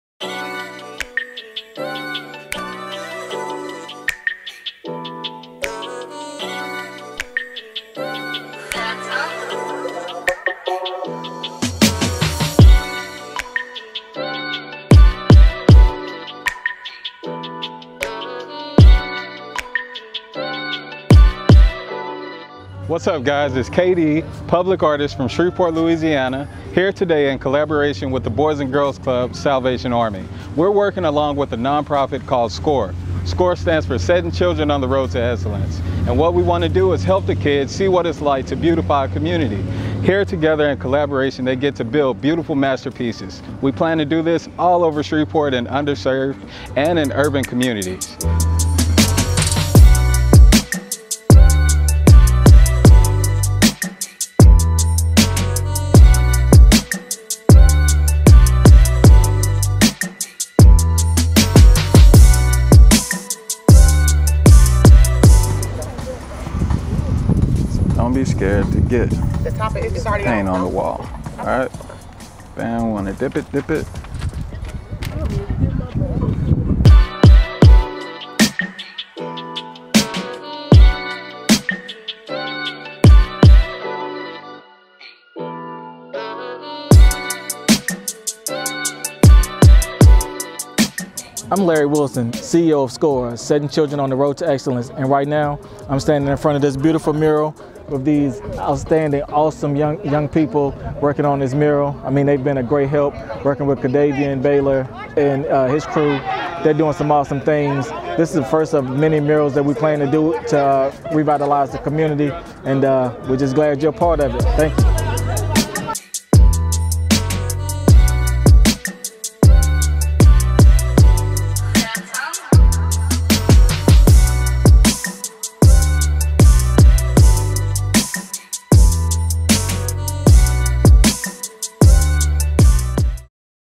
S.C.O.R.E. Public Art Campaign 2020-2021
S.P.A.C. (S.C.O.R.E. Public Art Campaign) is a revitalization initiative to foster youth development through public art projects in several under served communities of Shreveport, Louisiana by local nonprofit SCORE (501C3) and public artist KaDavien Baylor.

What better way to get the community involved in public art than having the community help fund AND create the project.